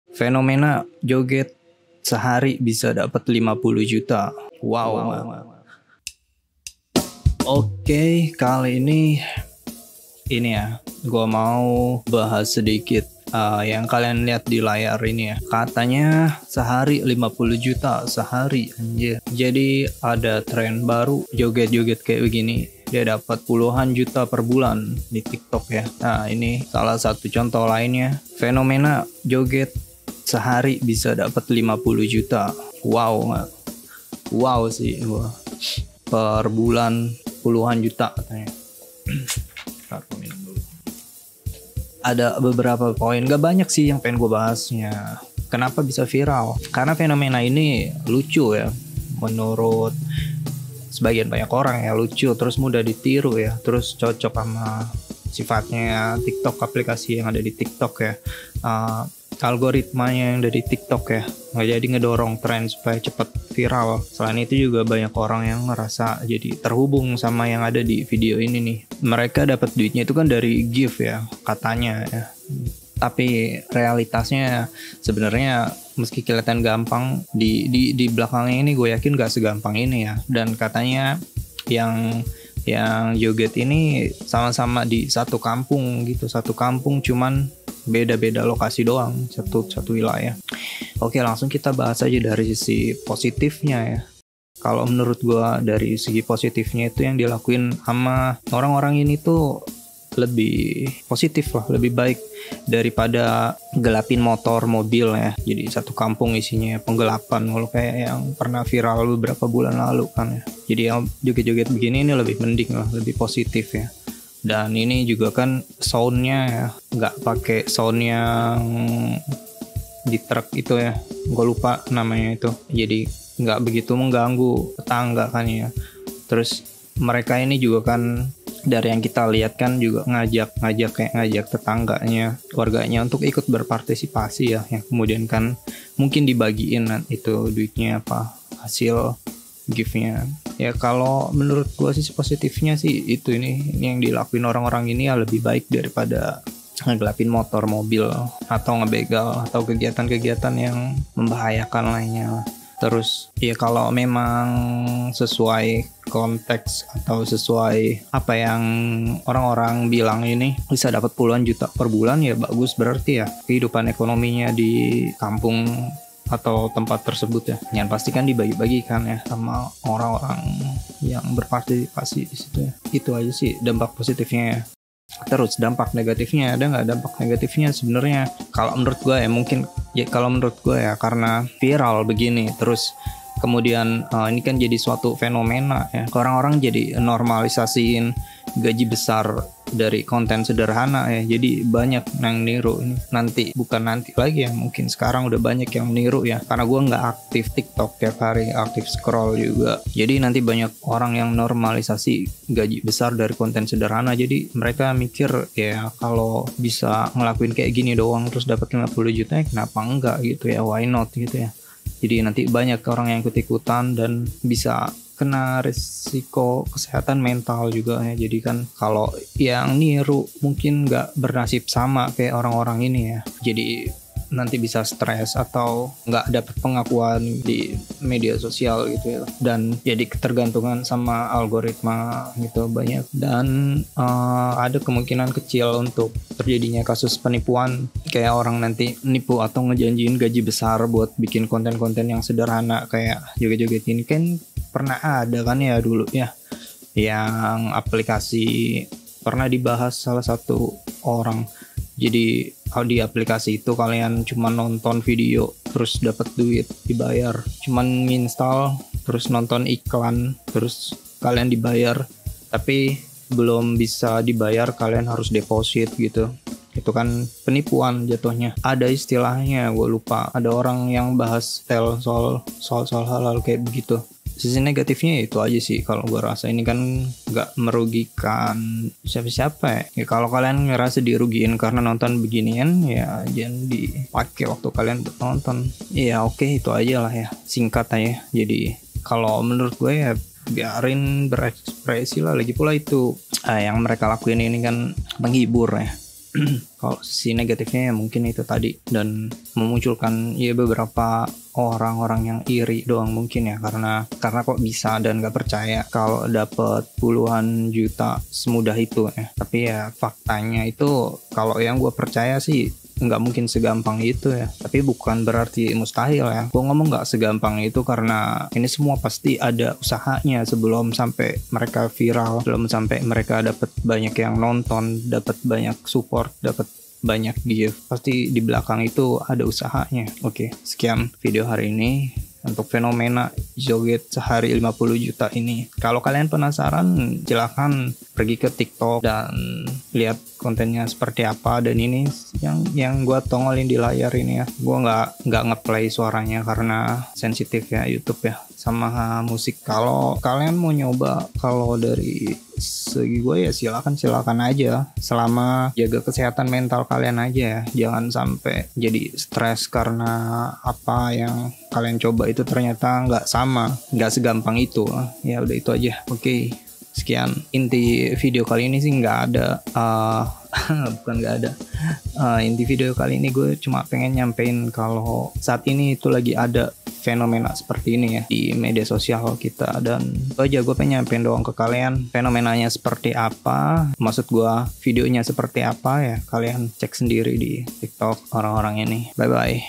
Fenomena Joget Gaji 50 Juta YANG VIRAL!! 🤯
Fenomena Joget Dapat 50 Juta di TikTok | Gimmick atau Beneran yak?

Gokil banget nggak sih? Belakangan ini rame banget di TikTok orang yang cuma joget-joget bisa dapet gaji sampai 50 juta! Apakah beneran bisa kaya cuma dengan joget? Atau ini cuma gimmick biar viral? Di video ini gue bakal bahas fenomena ini, kenapa bisa viral, dan sebenernya seberapa realistis sih klaim ini?

Like kalau lo juga penasaran sama fenomena ini, dan share ke temen lo yang suka joget di TikTok coy! 💃🔥🕺